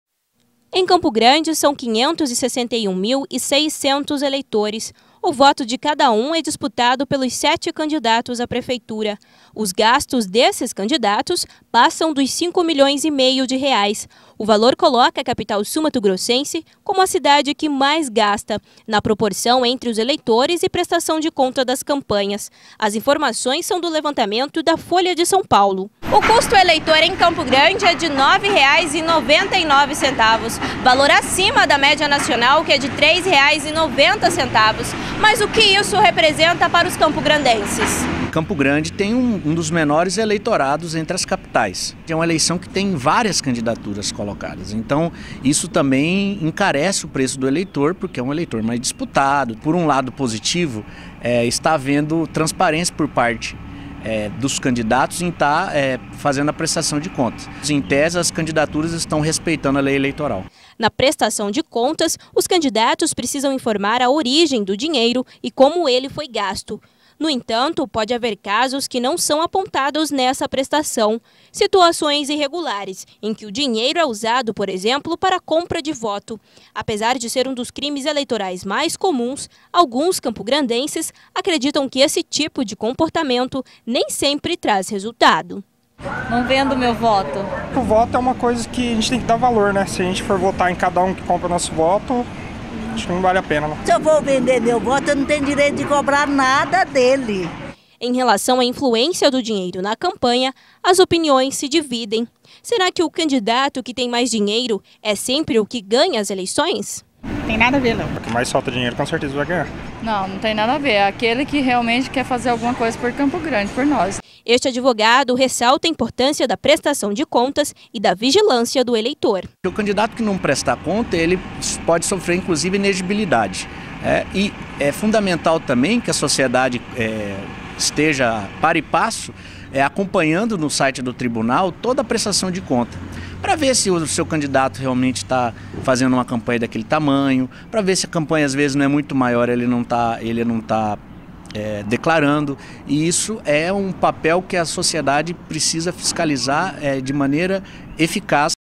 110912 PESQUISA INDICA QUE CUSTO ELEITOR DE CAMPO GRANDE É O MAIOR DO BRASIL
Pesquisa nacional indica que, nesta campanha, o custo do eleitor em Campo Grande é o mais alto do Brasil. A pesquisa se baseia na declaração de gastos dos candidatos.